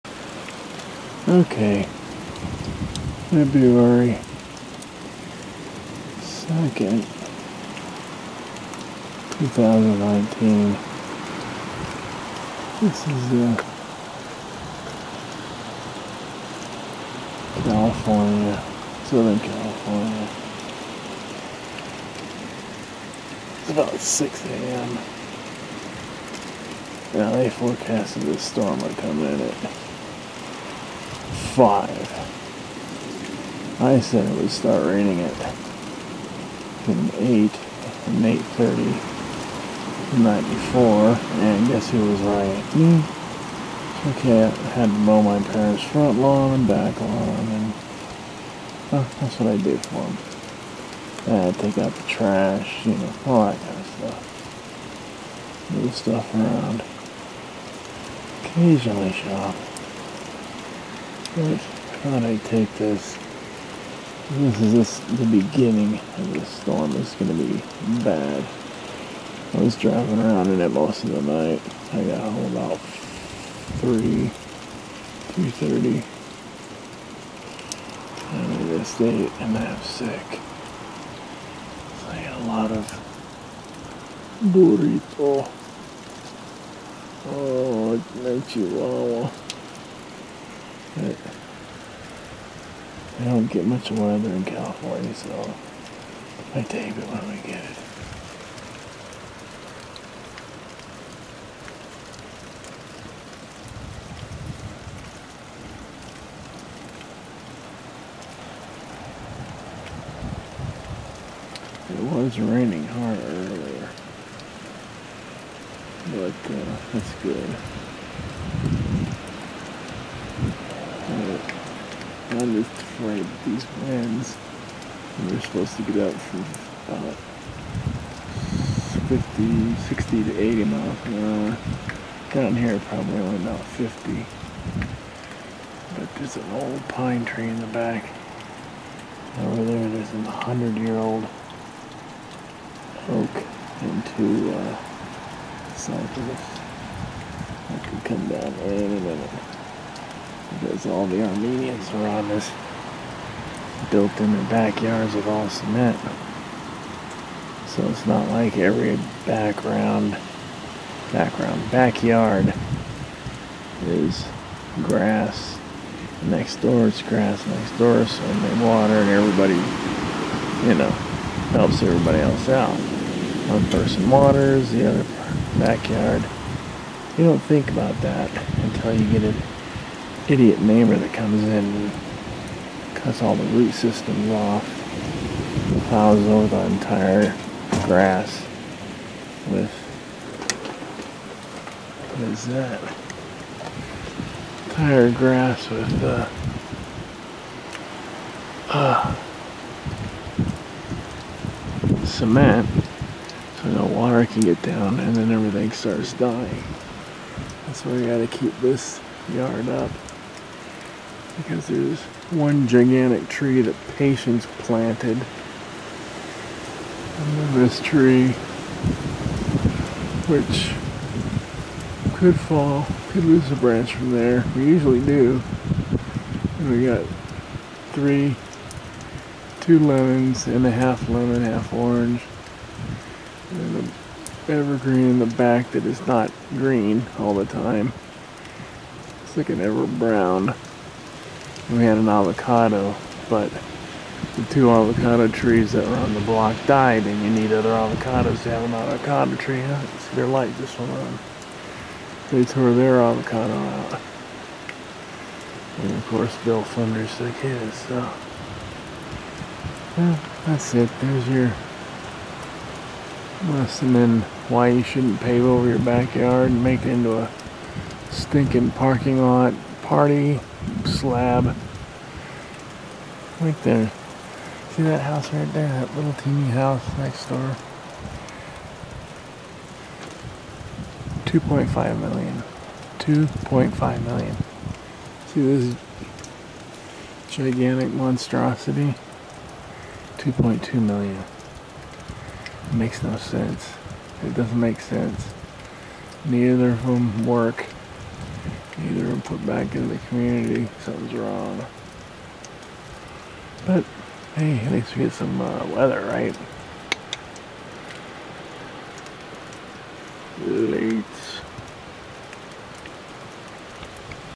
BEGINNING OF STORM BURBANK CA. 6 A.M. DAY 1 OF 7 PART 1 ...
JUST SHOT A FEW MINUTES OF RAIN,2 HOURS LATER WAS A BIG THUNDER STORM . THE NEXT VIDEO IS FROM THE NATL. WEATHER.SERVICE. OF A TORNADO WARNING!! THAT IS ON PART 2!